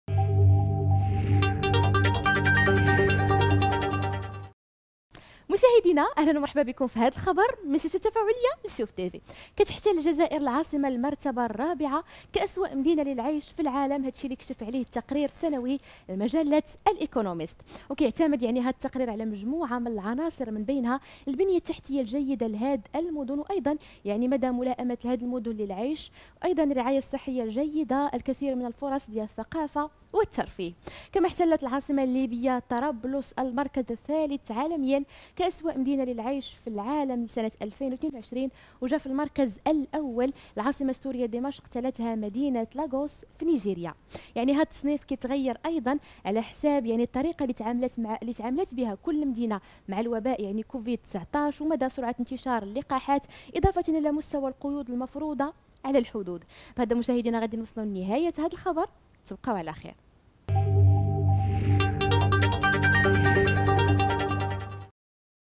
الجزائر العاصمة رابع أسوء مدينة للعيش في العالم
كشف التقرير السنوي لمجلة "الإيكونوميست" عن احتلال الجزائر العاصمة للمركز الرابع كأسوأ مدينة للعيش.